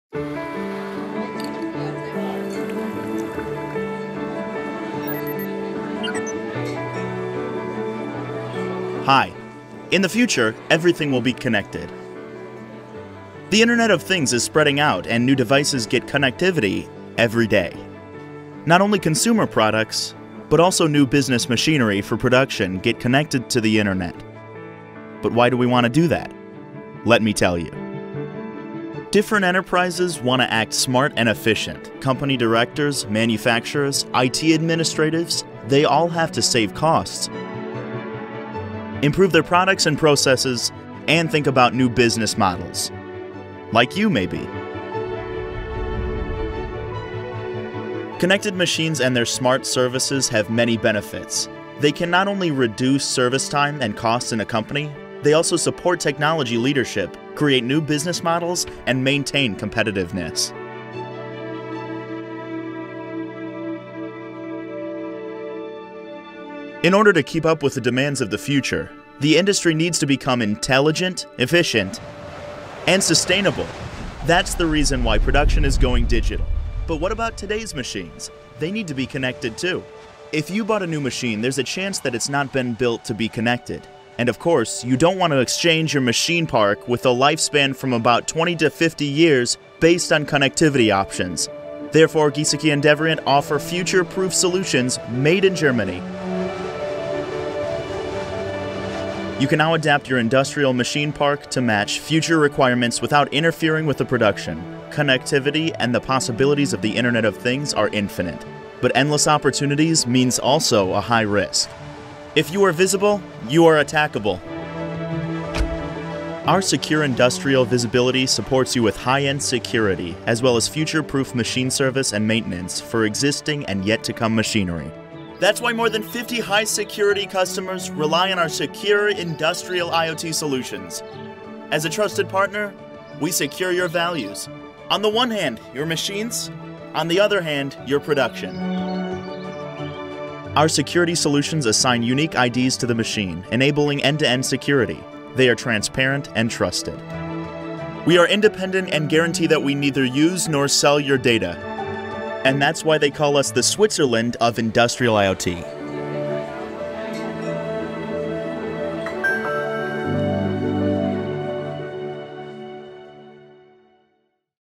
G+D enables industrial connectivity
The IoT is spreading rapidly. New devices are connected every day - not only the consumer products most of us use every day, but also industrial machinery and infrastructure. But why is being part of the IoT important to industrial enterprises? Companies today must be smart and efficient. They focus on cost savings, improved products and processes and new business models. Connected machinery plays an essential role in improving productivity and efficiency. G+D enables industrial connectivity – and wraps it in state-of-the-art security.